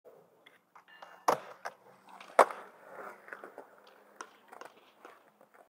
Bell ringer - Nollie pressure flip rise!
Skateboard with purpose, join us through our ups and down as we complete our ultimate tricklist.